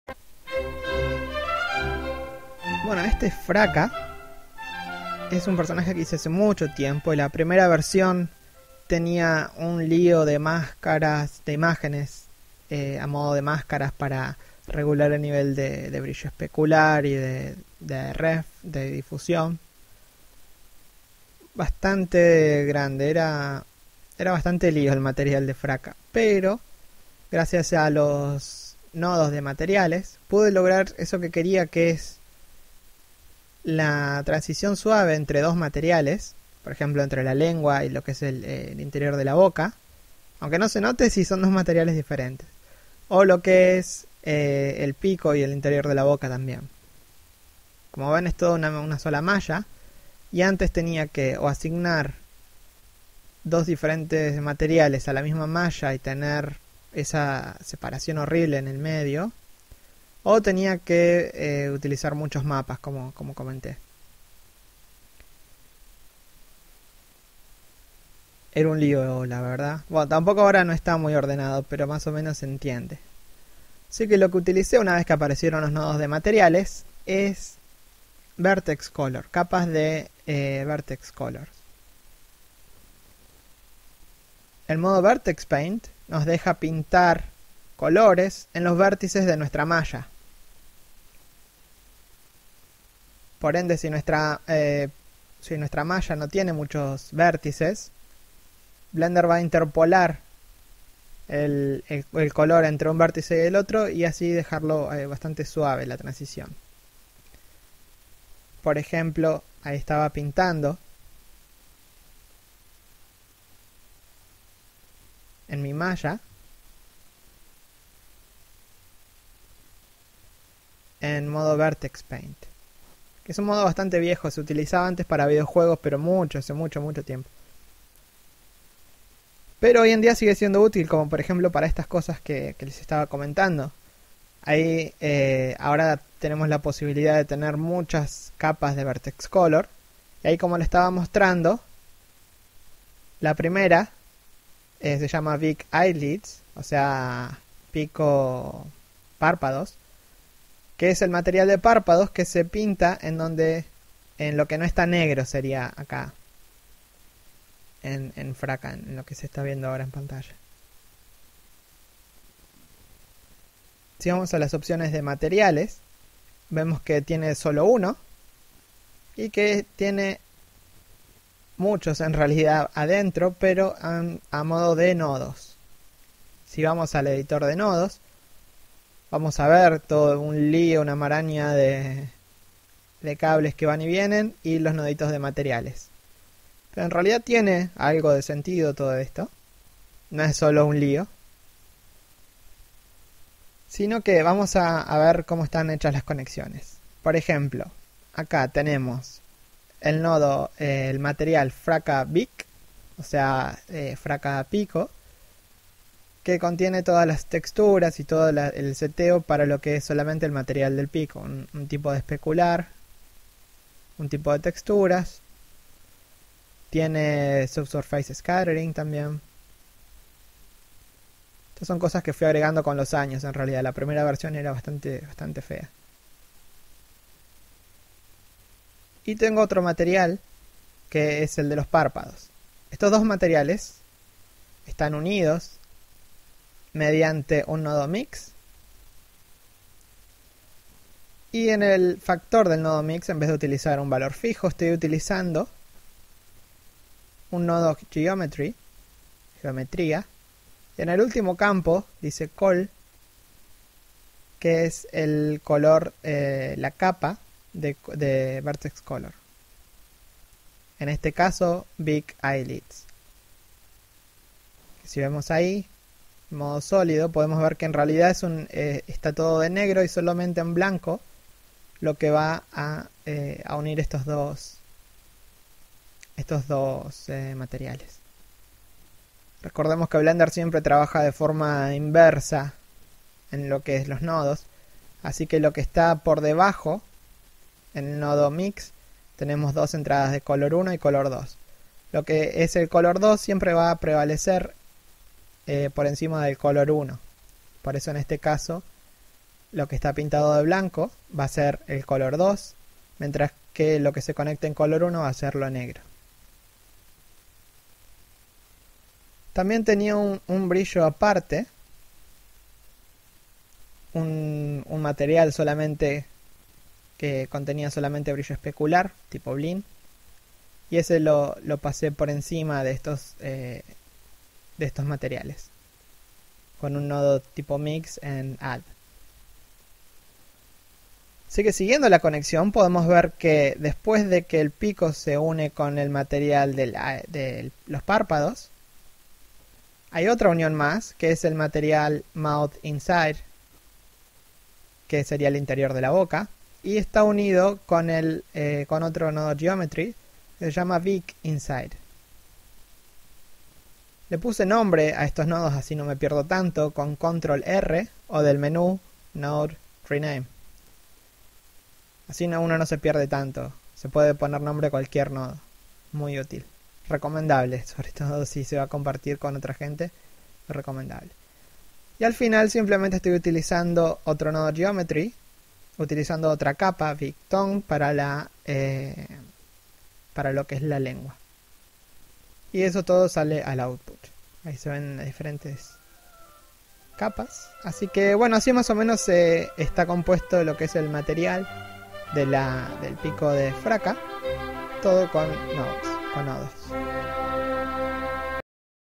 Venom's Lab! Español: 04.03 - Fraka's Nodes
Blender Foundation's Training DVD #4, Venom's Lab!.

Usando Blender 2.49b, pero es aplicable a las versiones actuales sin necesidad de muchos cambios.

Audio en Castellano.